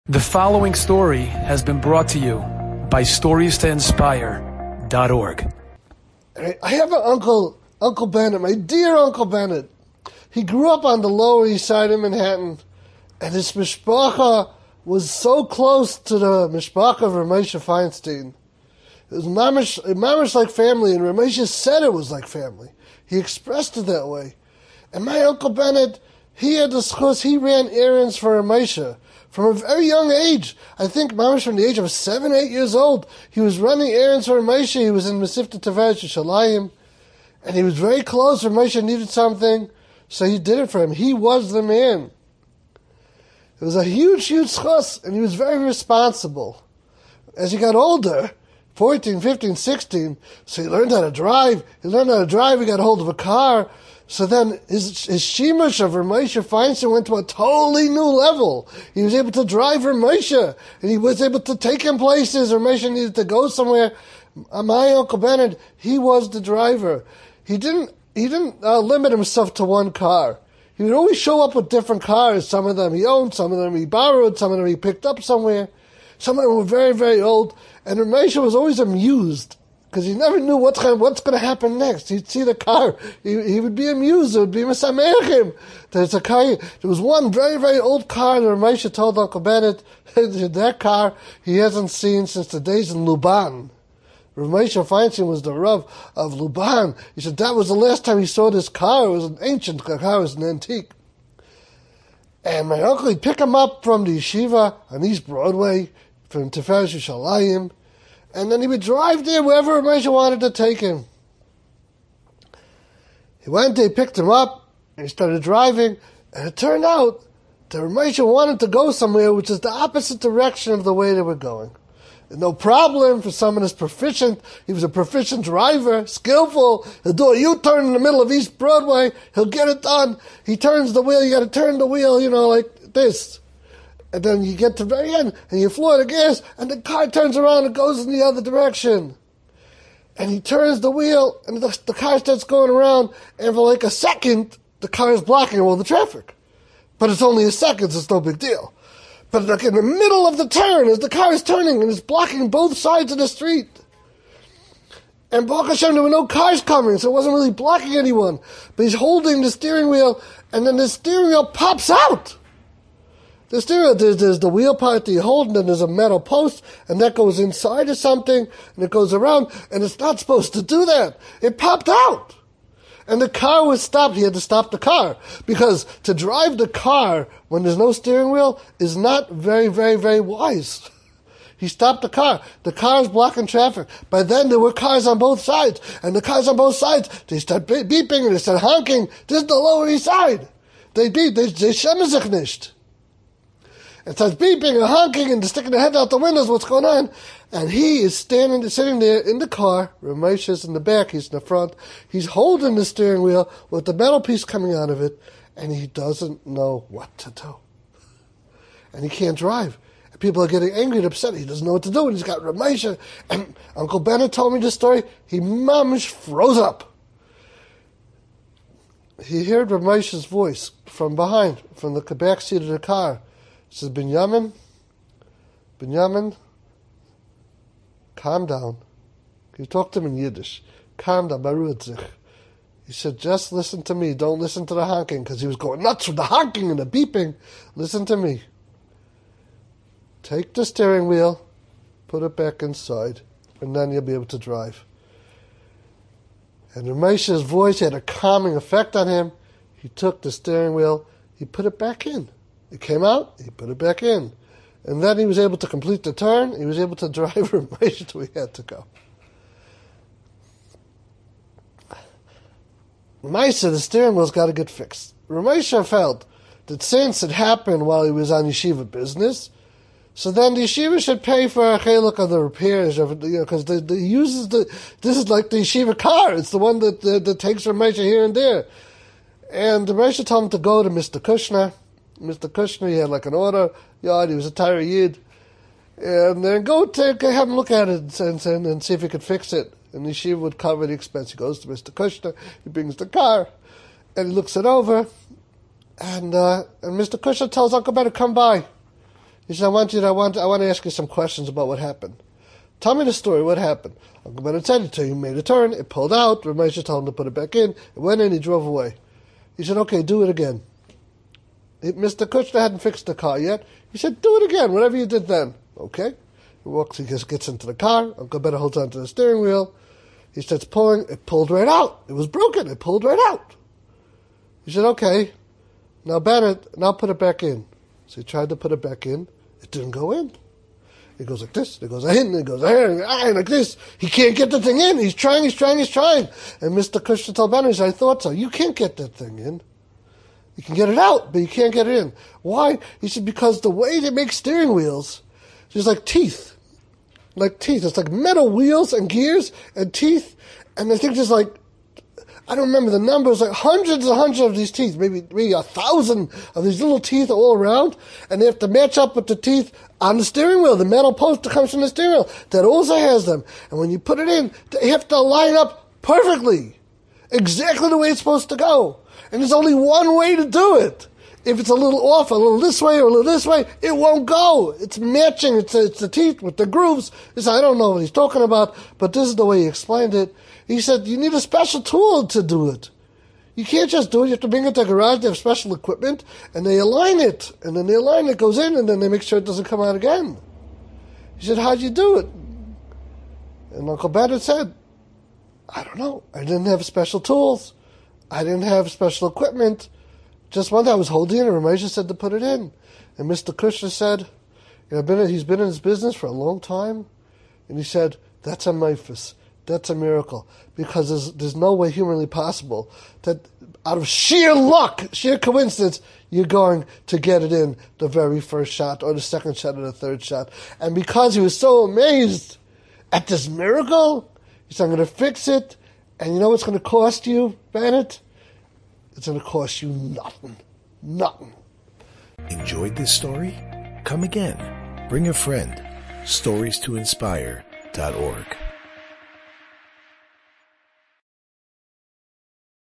The Miraculous Steering Wheel - A Story About R' Moshe Feinstein - Rabbi Pesach Siegel
R' Pesach Siegel

👀 More videos from this speaker